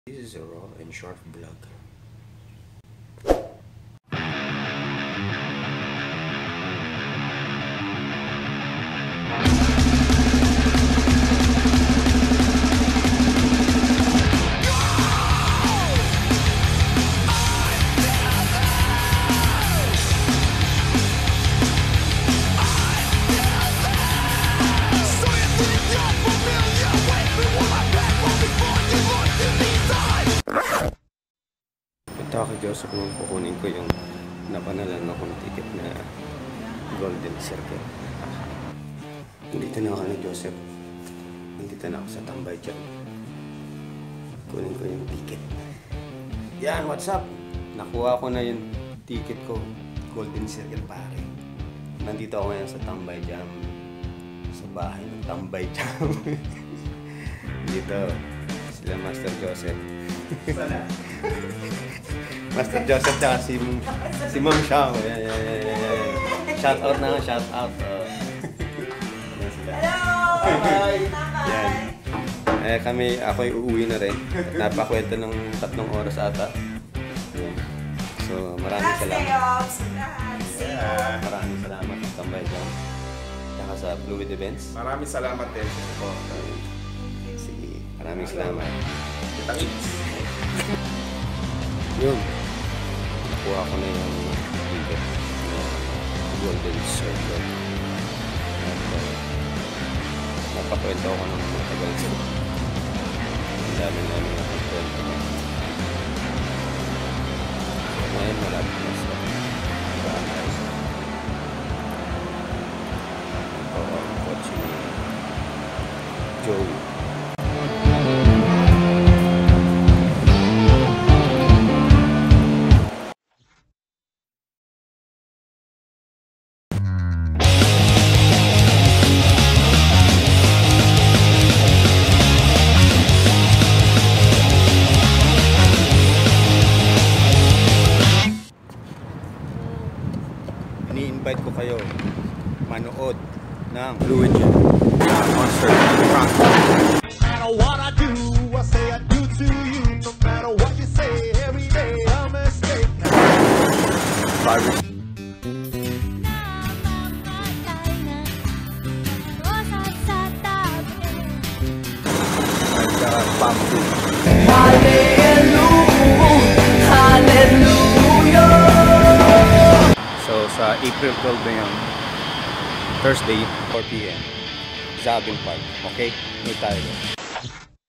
PROJECT WINNER: Maraming salamat Tambay Jam at Fluid Events
Thank you very much Tambay Jam and Fluid Events for the free Golden Tickets.

FLUID JAM:  FRANCO, BARBIE and BAMBOO
LIVE AT THE ZAABEEL PARK